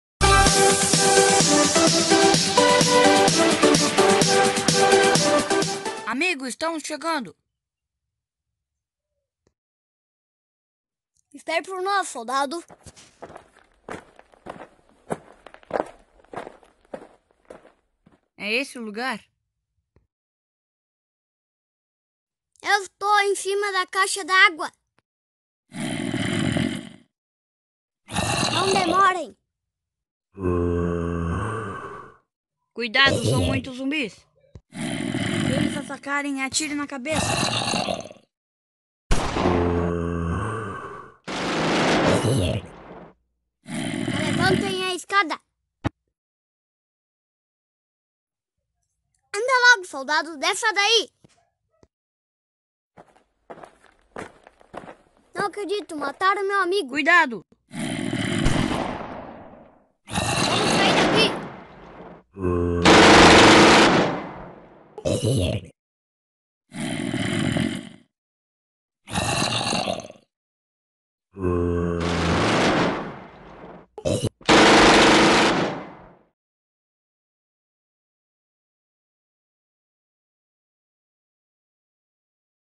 Stop Motion - Soldadinhos x Zumbis
Em uma missão de resgate os Soldadinhos vão ter que salvar seu amigo preso em cima da caixa d'água cercado de zumbis...